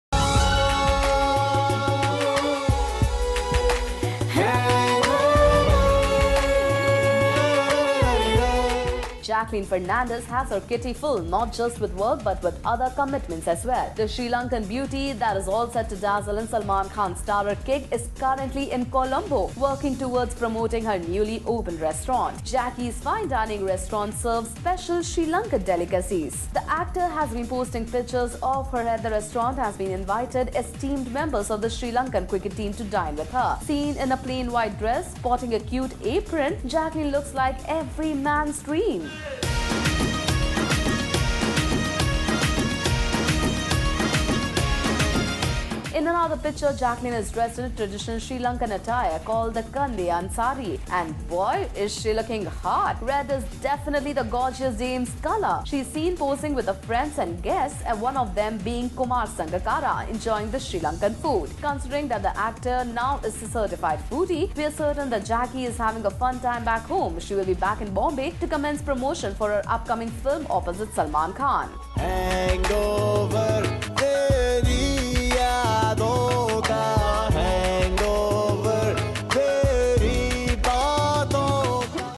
Juggling Promotions and More | Bollywood Masala | Latest Bollywood News
Watch Latest Bollywood Masala News The actor who will be seen romancing Salman soon is currently juggling promotions and more
Jacqueline Fernandez has her kitty full not just with work but other commitments as well. The Sri Lankan beauty who is all set to dazzle in Salman Khan starrer Kick is currently in Columbo working towards promoting her newly-opened restaurant. Jackie’s fine-dining restaurant serves special Sri Lankan delicacies. The actor has been posting pictures of herself at the restaurant and has even invited esteemed members of the Sri Lankan cricket team to dine with her. Seen here in a plain white dress sporting a cute apron, Jacqueline looks like every man’s dream! In another picture, Jacqueline is dressed in traditional Sri Lankan attire called the kandyan saree and boy is she looking hot! Red is definitely the gorgeous dame’s color! She is seen posing with her friends and guests (one of them being Kumar Sangakkara) enjoying the Sri Lankan food.